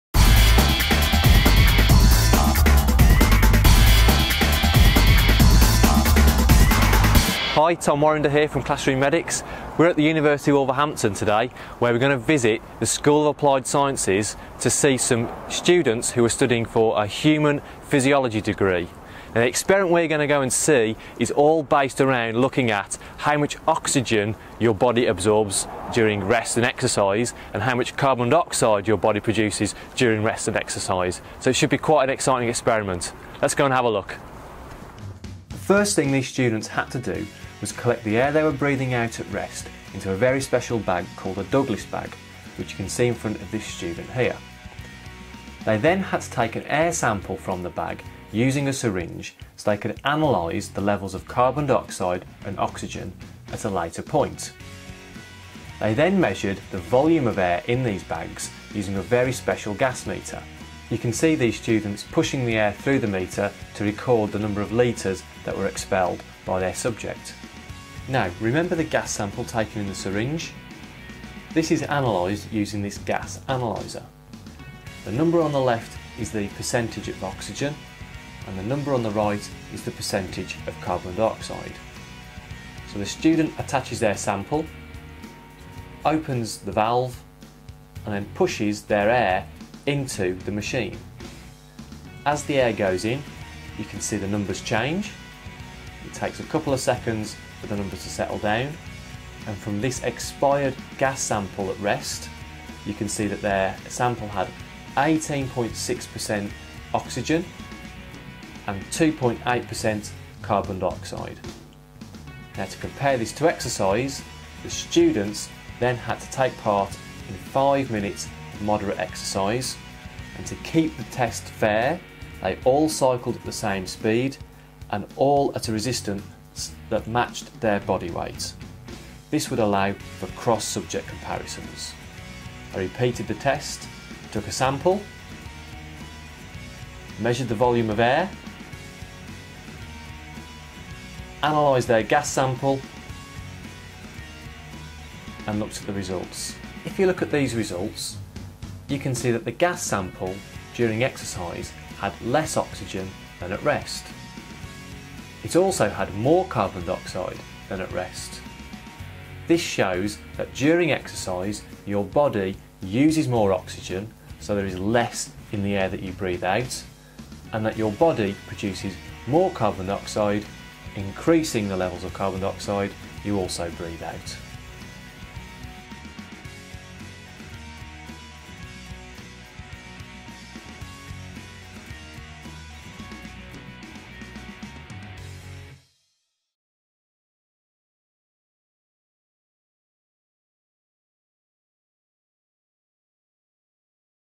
Human Physiology Degree
A brief look at an experiment from the University of Wolverhampton's Human Physiology Degree. The experiment looks at how much O2 you use and how much CO2 you produce at different levels of exercise. To find out more about this course